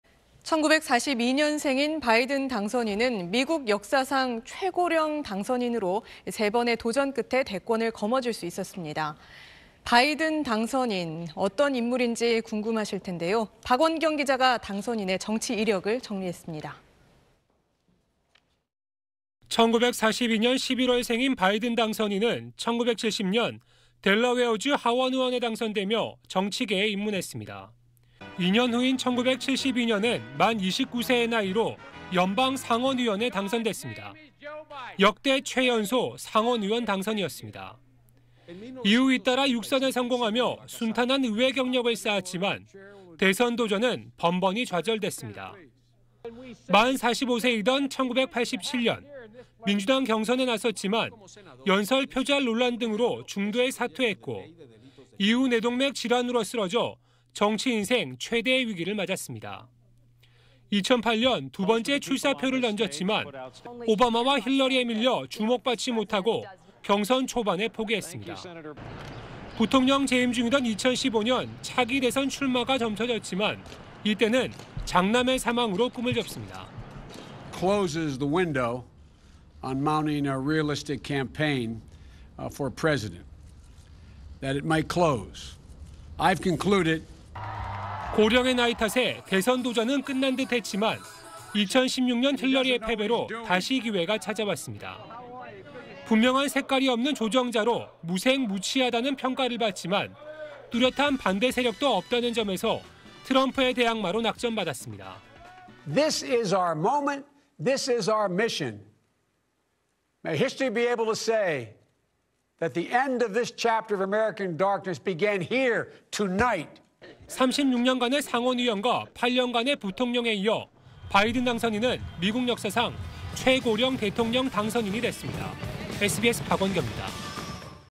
바이든, 3수 만에 거머쥔 대권…'만78세' 역대 최고령 / SBS
1942년 생인 바이든 당선인은 미국 역사상 최고령 당선인으로, 3번의 도전 끝에 대권을 거머쥘 수 있었습니다. 바이든 당선인, 어떤 인물인지 궁금하실 것입니다.
박원경 기자가 당선인의 정치 이력을 정리했습니다.